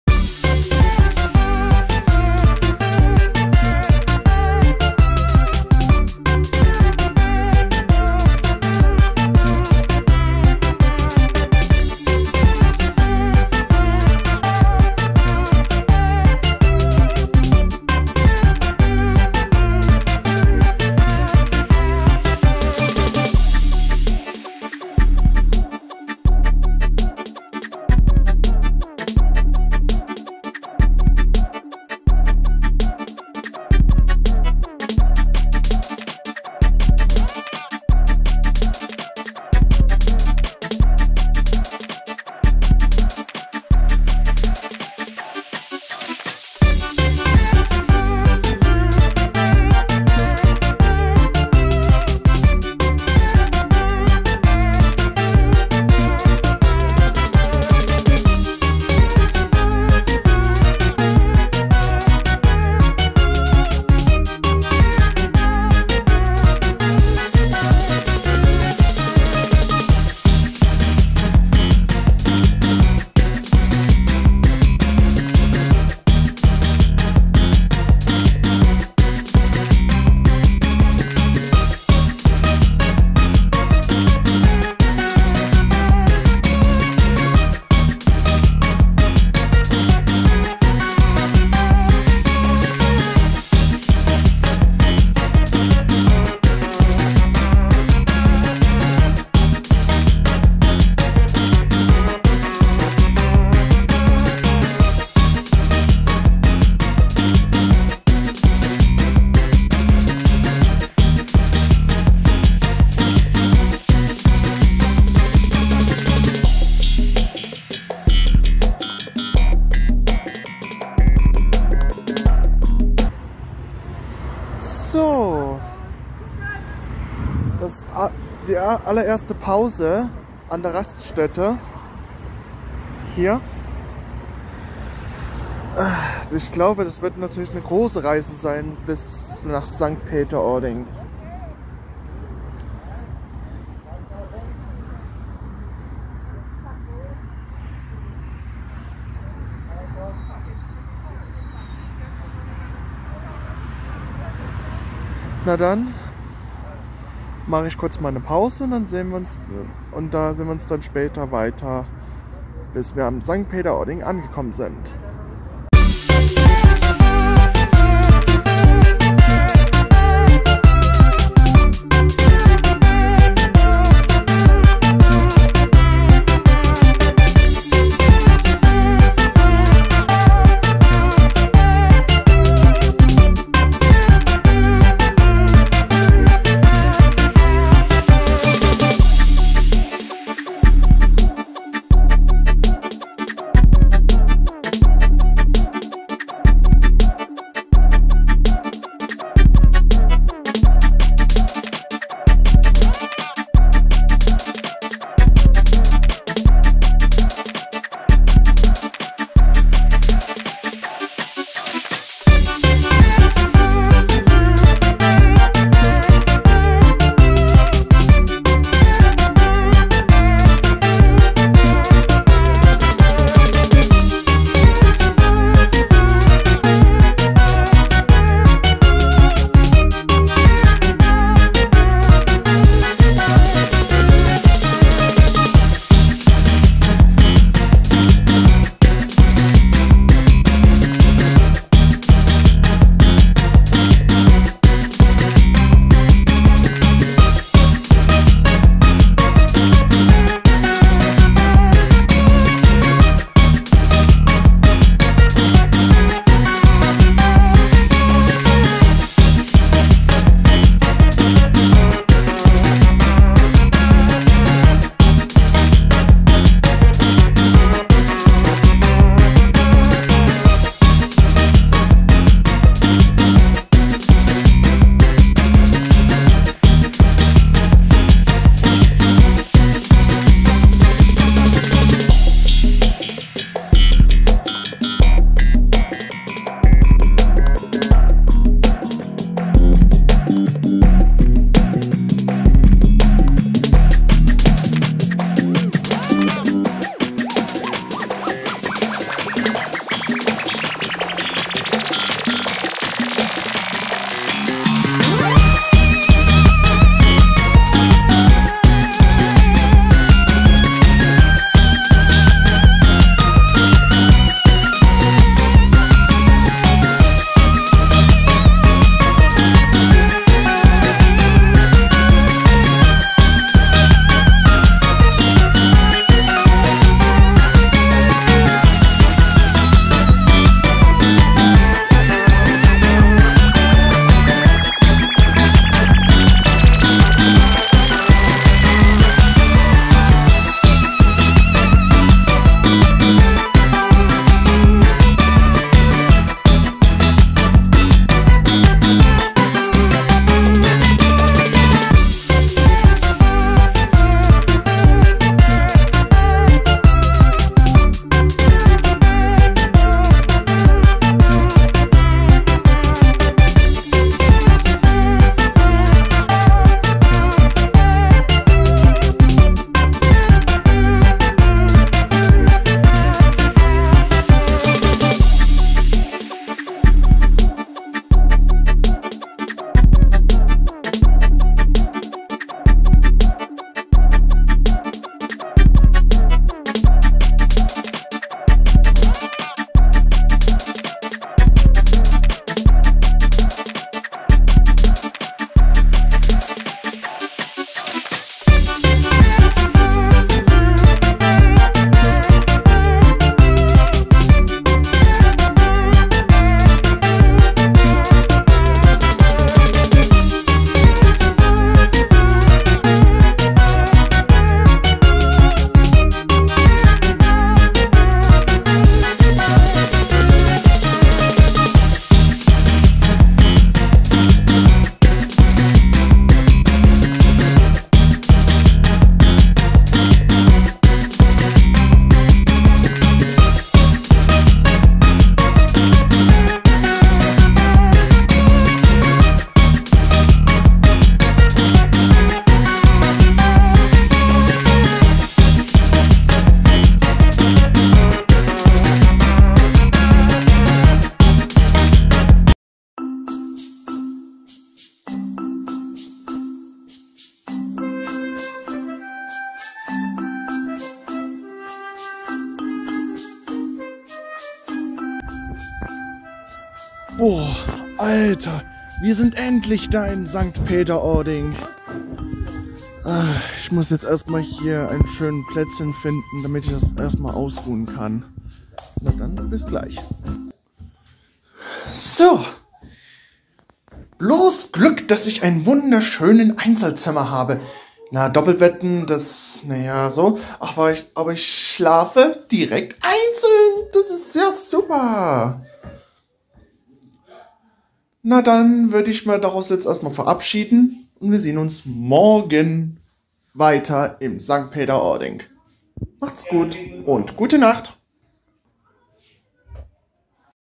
Tag 1: Reise von Berlin bis Nordsee
Die Große Reise beginnt! Berlin, Hamburg bis St. Peter Ording